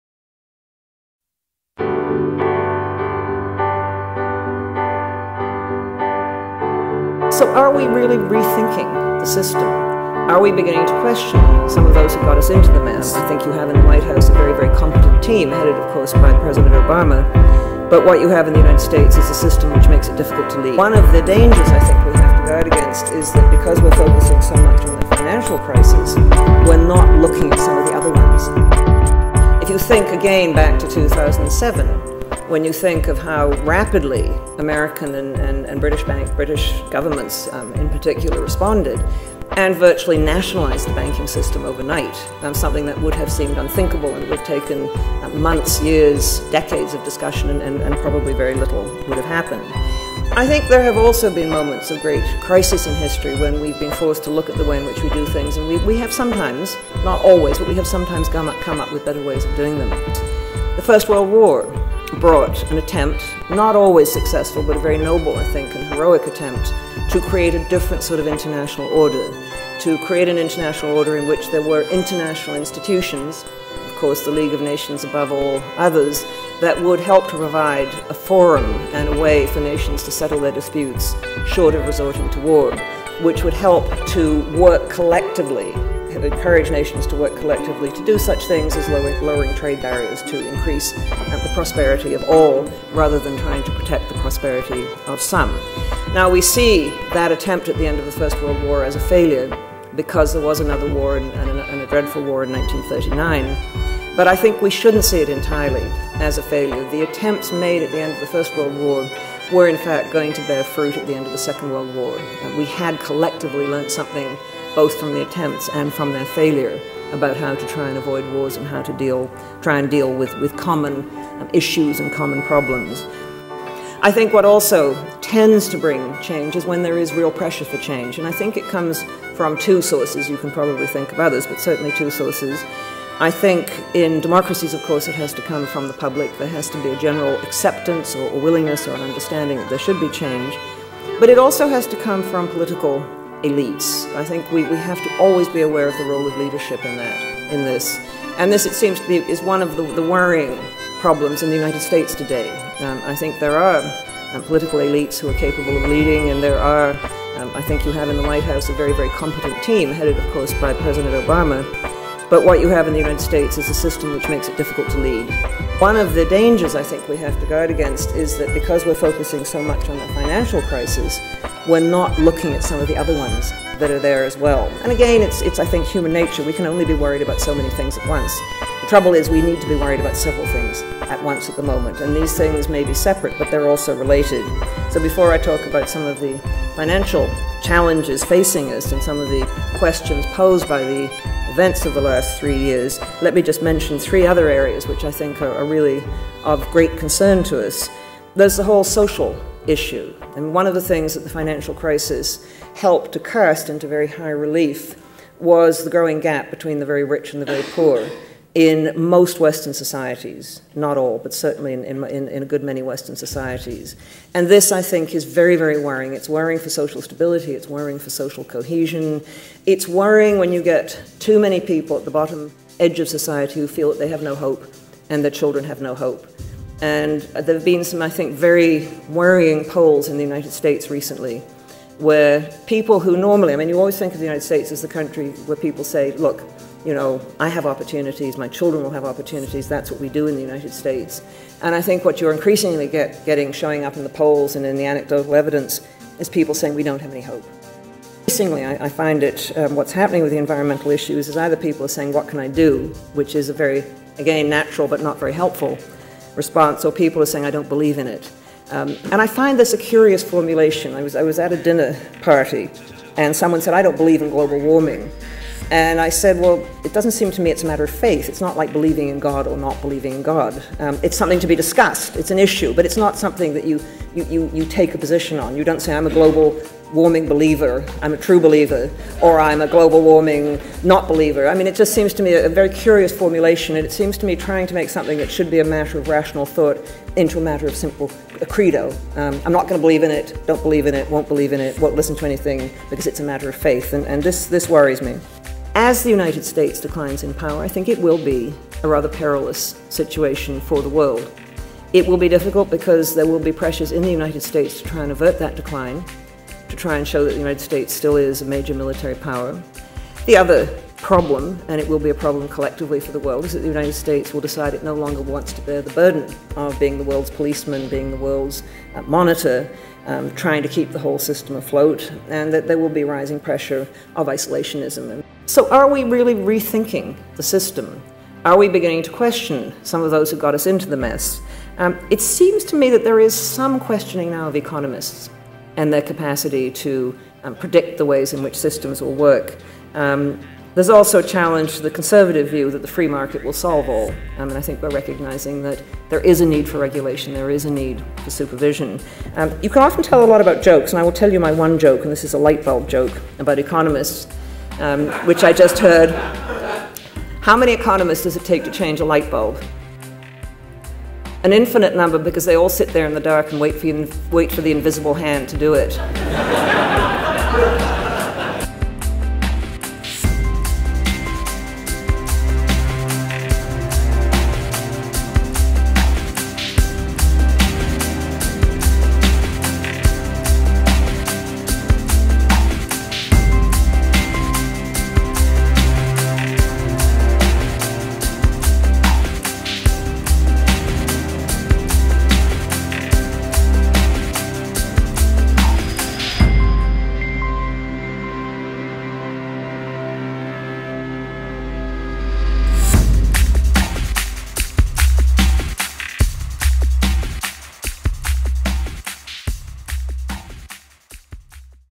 Highlights: Opening Keynote of the 2010 Couchiching Summer Conference
Watershed Moment or Wasted Opportunity

Margaret MacMillan (Author, Paris 1919)

Couchiching Institute on Public Affairs
couch.ca

Live Event Video Production: Dawghaus Studios (dawghaus.ca)